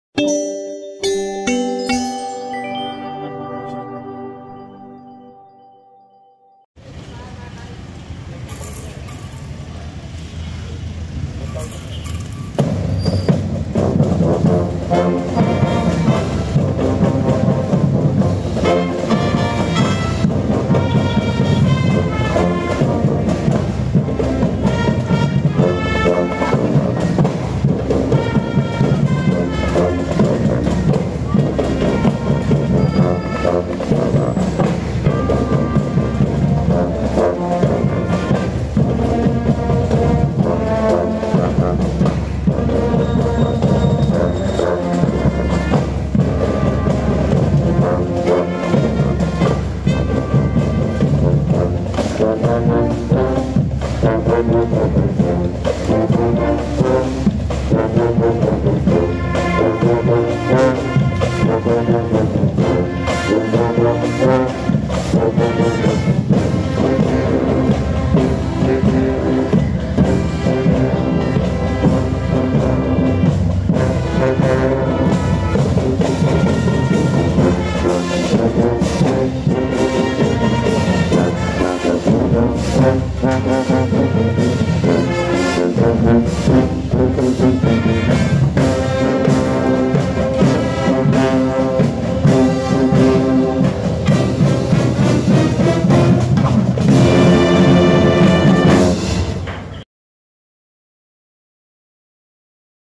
Bakood Festival 2024 I Band Rehearsal I Citizens Brigade Band of Dasmariñas
Bakood Festival 2024
Band Rehearsal
Citizens Brigade Band of Dasmariñas